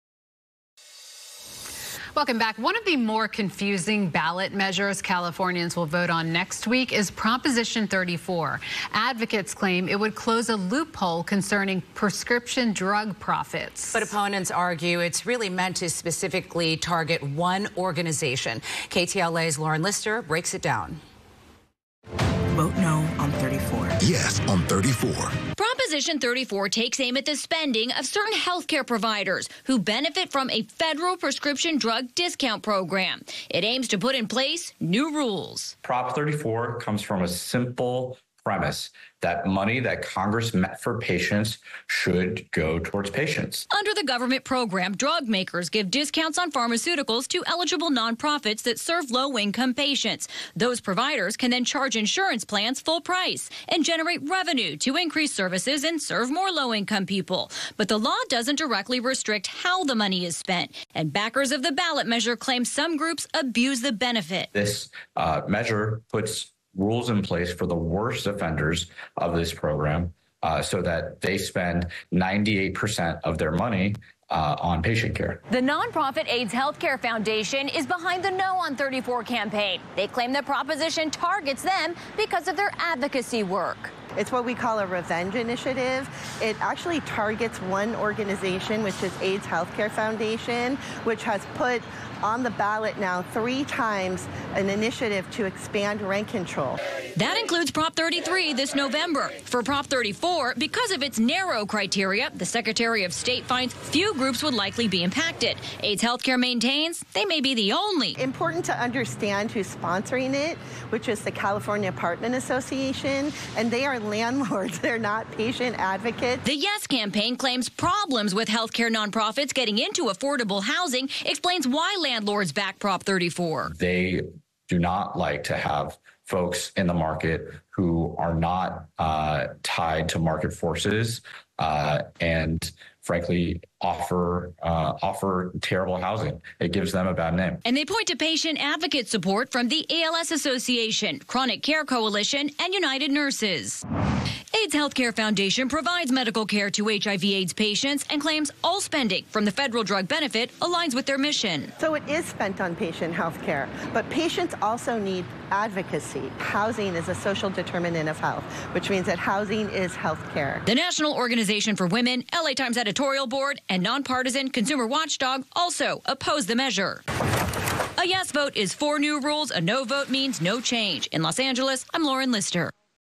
KTLA-LA (CW) - Los Angeles, CA: What To Know About California Prop 34
The National Organization for Women, LA Times editorial board, and nonpartisan Consumer Watchdog oppose the measure.